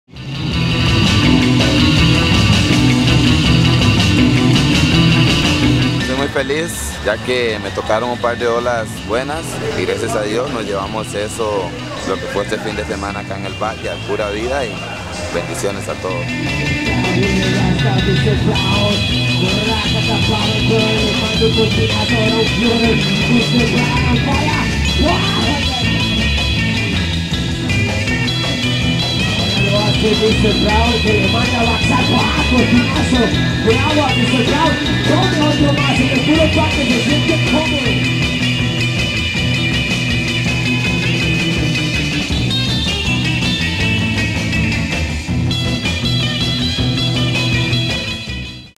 30 seconds with Gilbert Brown - Backyard Bar Surf Series 19/10/13
30 seconds with Gilbert Brown - Winner of the Backyard Bar Surf Series 19/10/13
Una producción de Wipeout Films.
Un video de Marco Monteroso